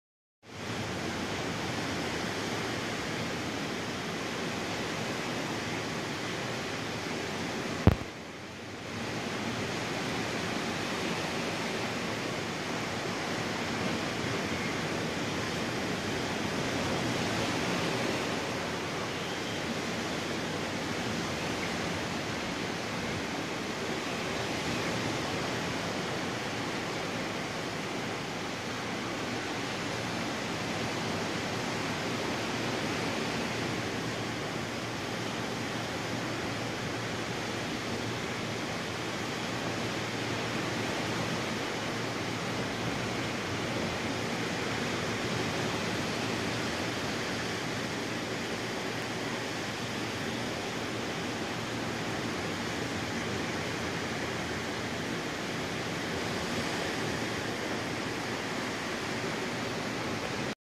A pleasant morning in April
Enjoying a pleasant April morning  on the balcony watching the waves roll in.